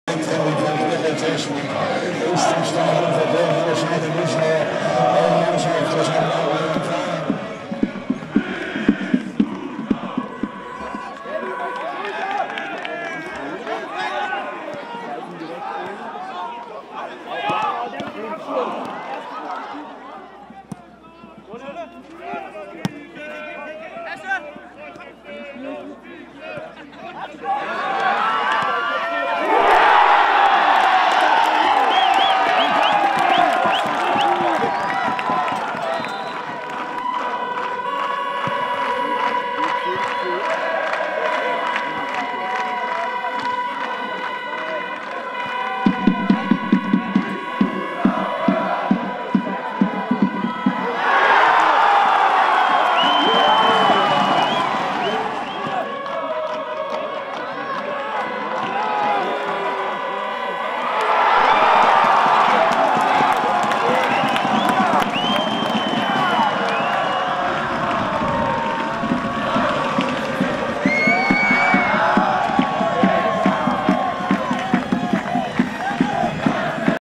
U19-Sachsenpokalfinale: Die Highlights zwischen Dynamo Dresden und Erzgebirge Aue | MDR
Dynamo Dresdens U19 hat das Ticket für den DFB-Pokal gelöst – dank eines verdienten Sieges im Sachsenpokalfinale gegen Erzgebirge Aue. Seht hier die Highlights der Partie auf einen Blick.